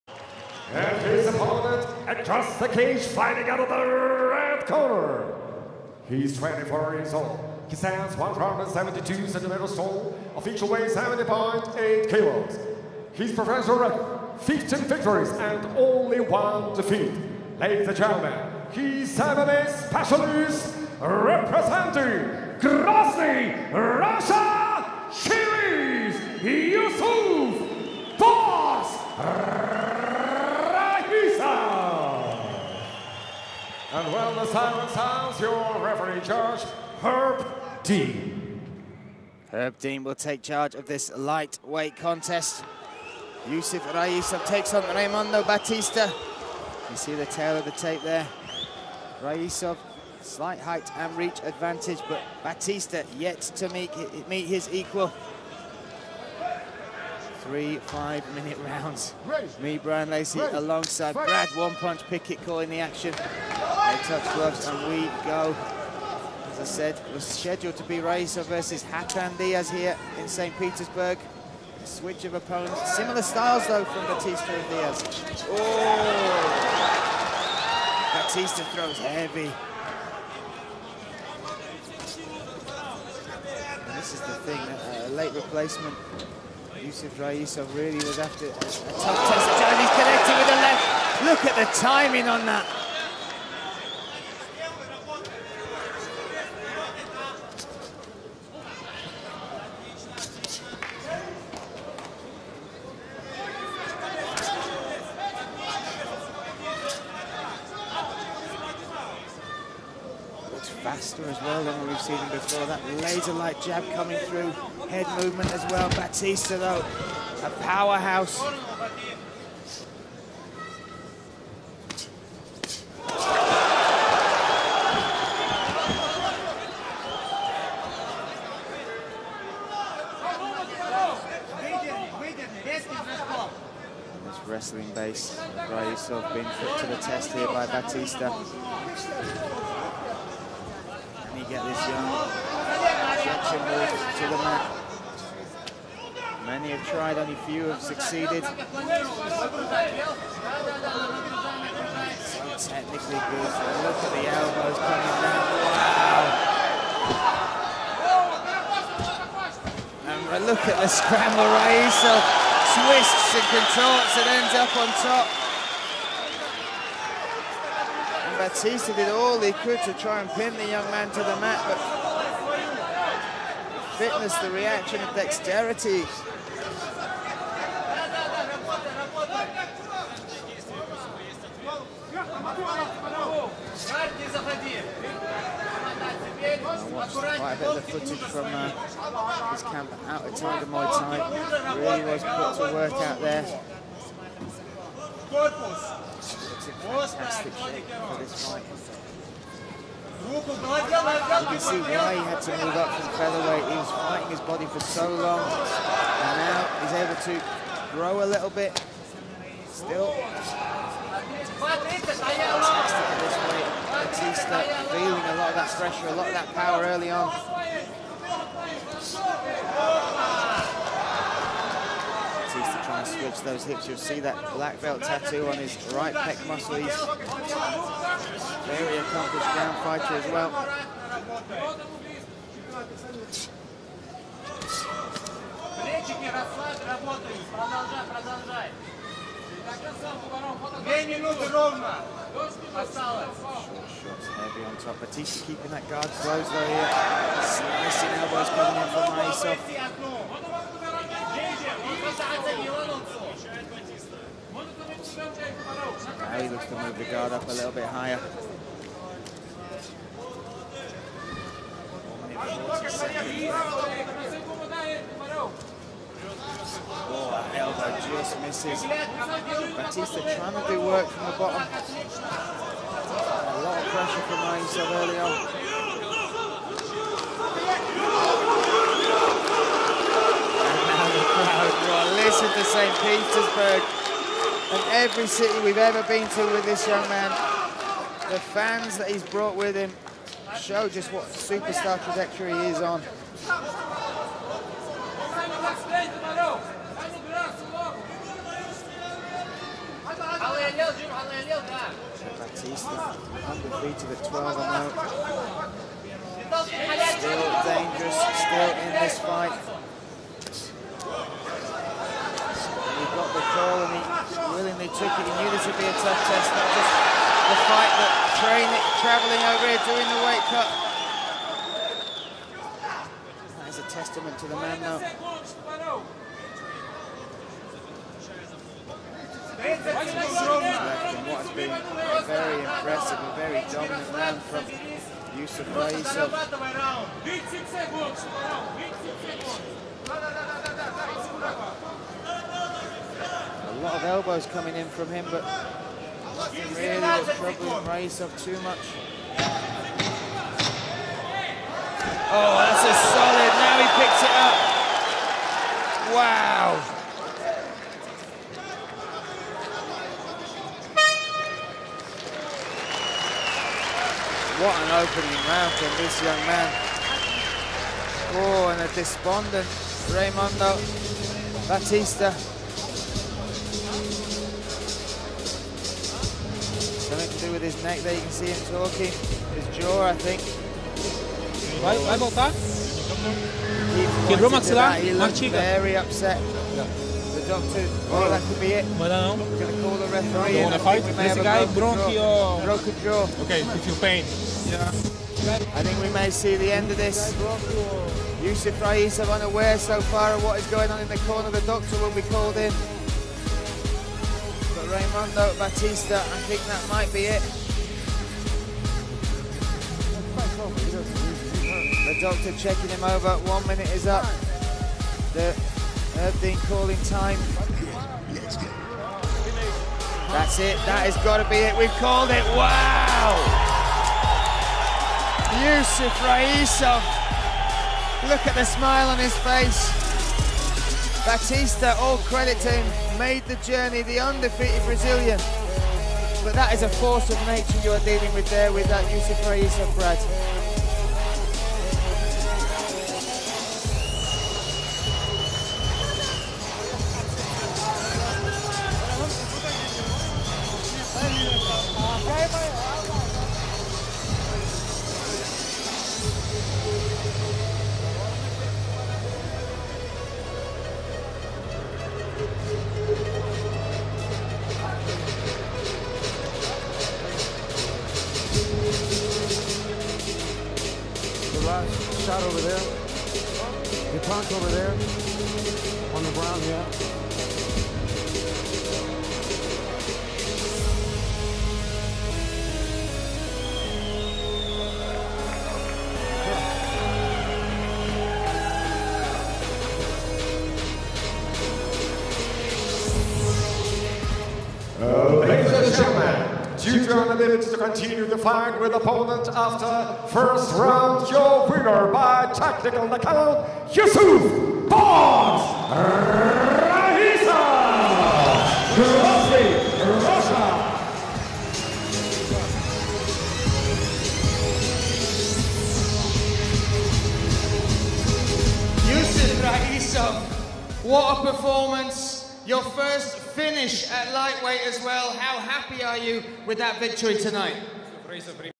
Raimundo Batista Vs Yusuf Raisov Knockout
Credits: ACA MMA
ACA 103 - Full fight: Raimundo Batista - Yusuf Raisov